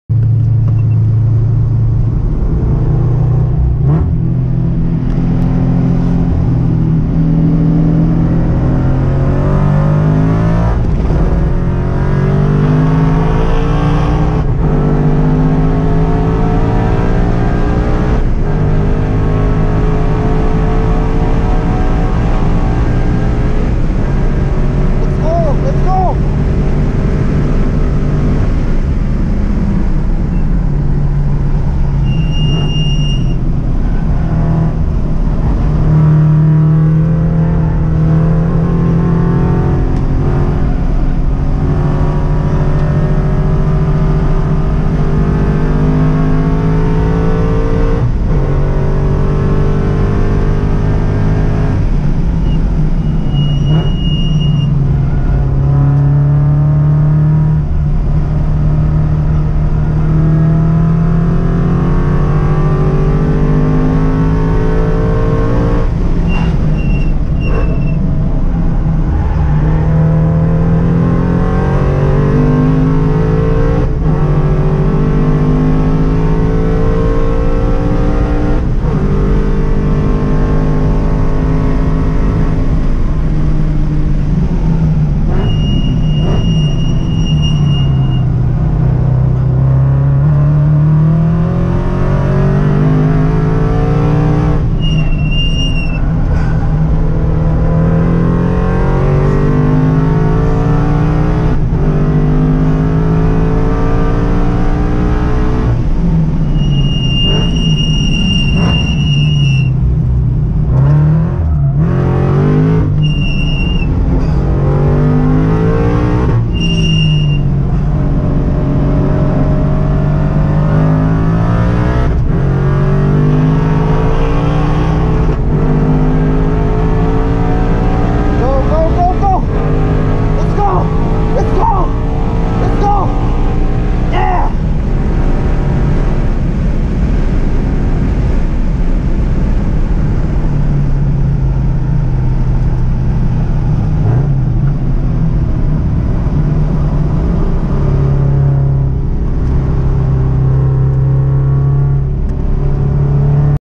Ridge motorsports park FRS (GT86) 1:50.6
RMP 1:50.6 on our 2015 frs.
During Ongrid time attack event.
On street tires, almost full interior car.